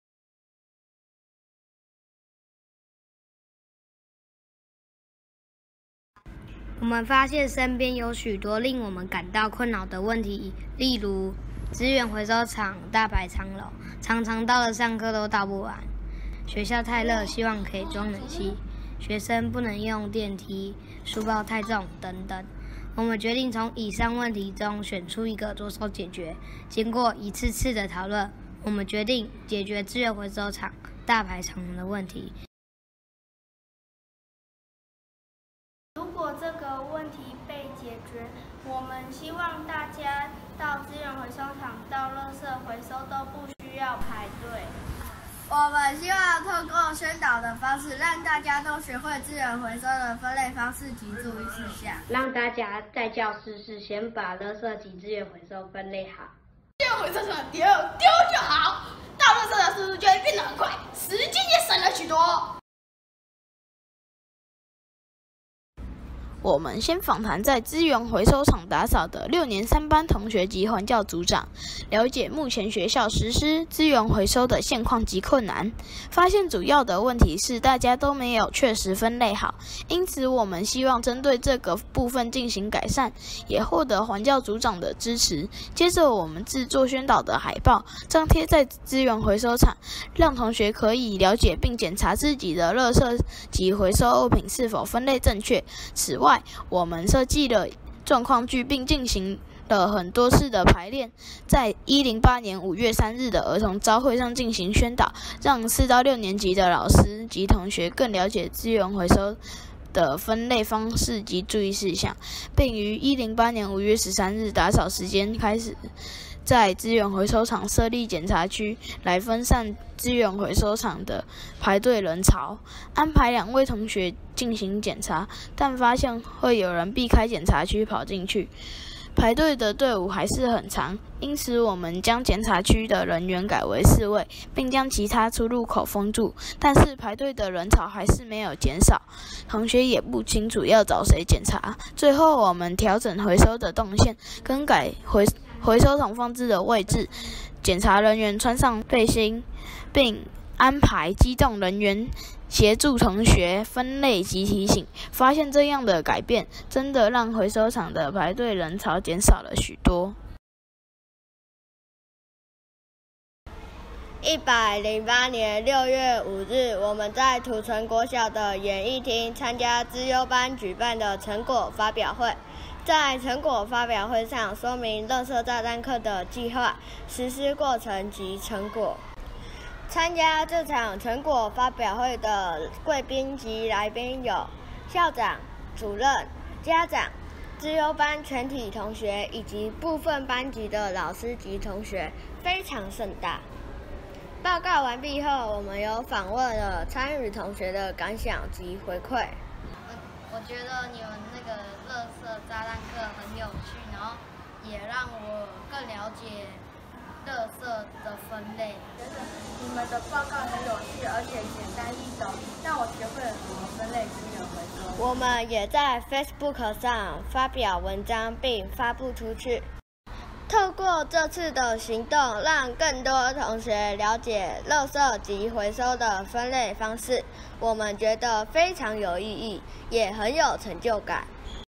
CHI-20200100 - 垃圾炸彈客
感受的問題：資源回收場每次都大排長龍，常常到了上課都倒不完。
輔導員： 邱奕、李思萱
小推手：
賴祈恩
陳弈劭
陳定璘
彭柏鈞